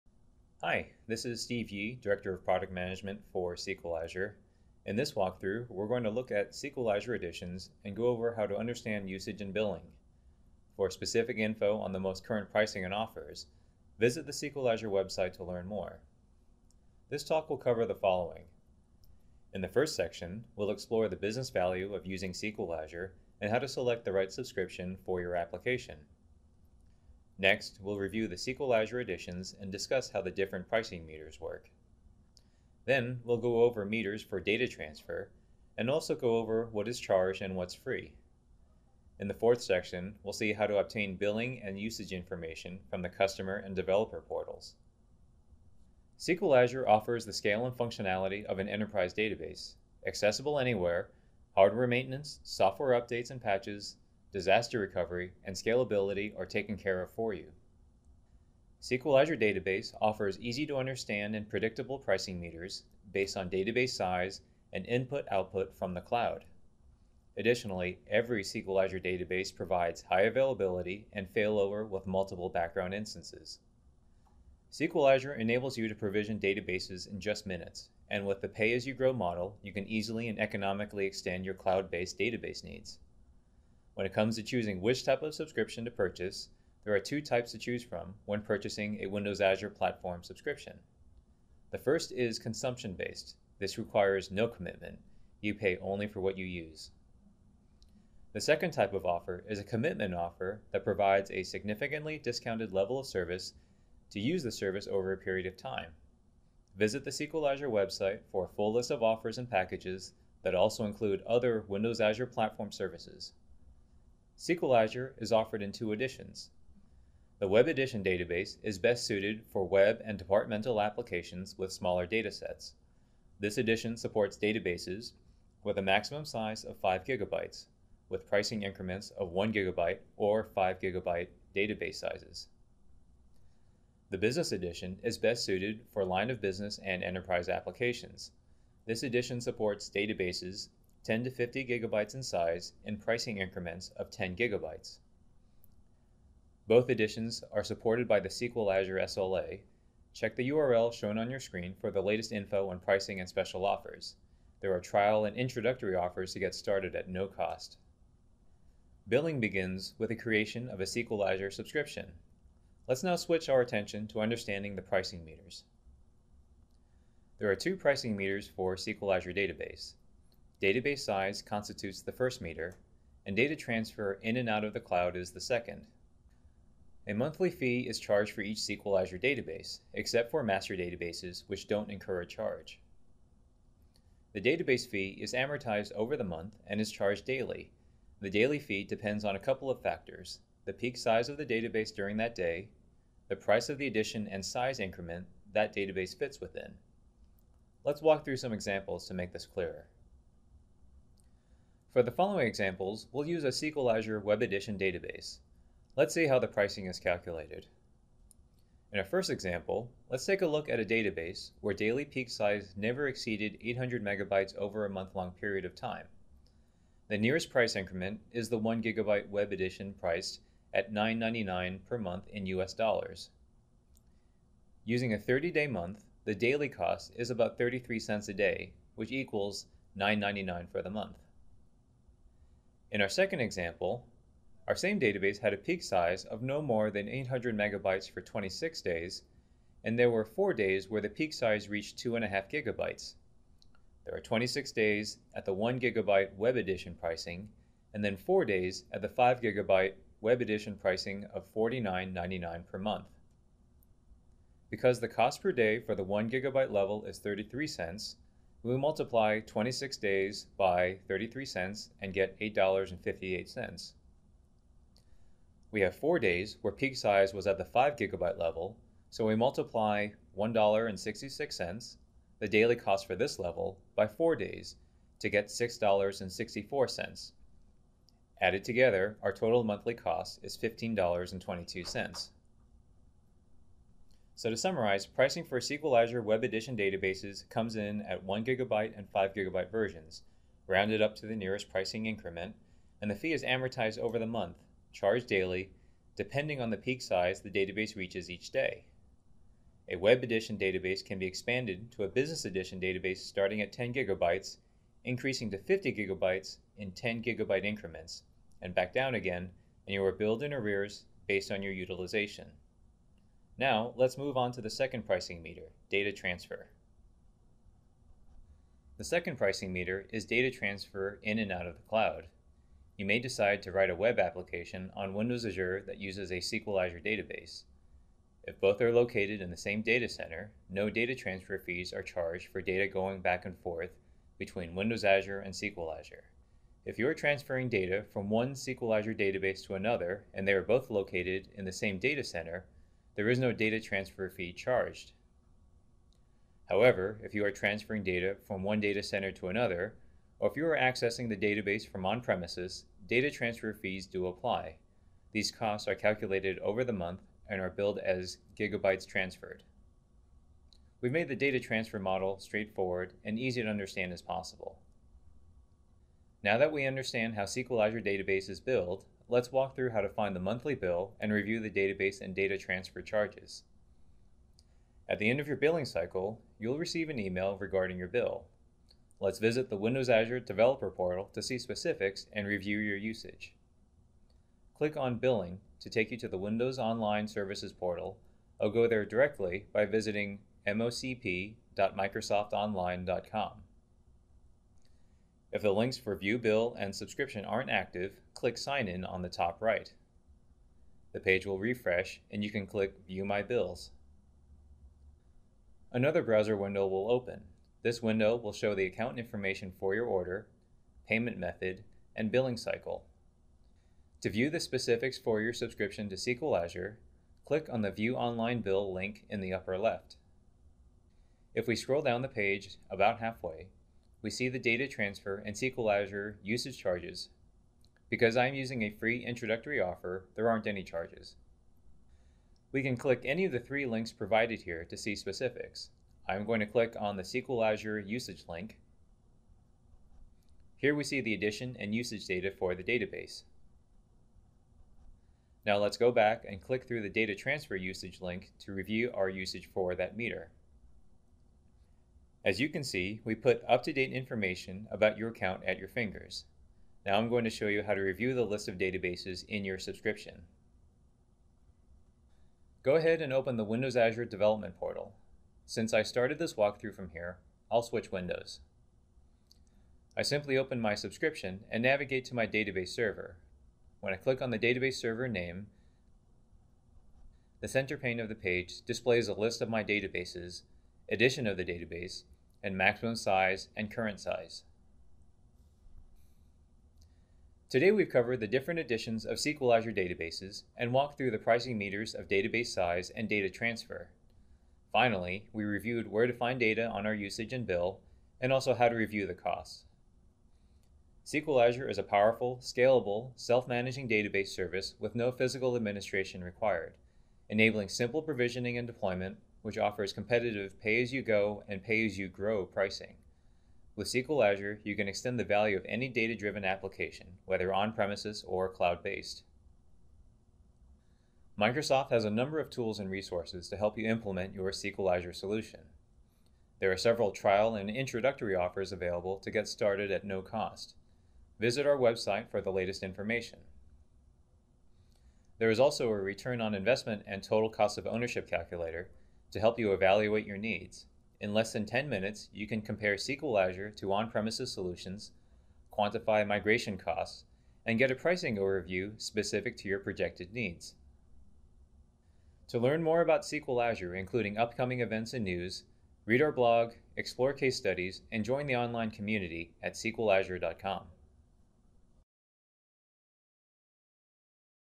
SQL Azure Pricing Explained
Watch this Video to understand the business value of SQL Azure and learn about usage and billing.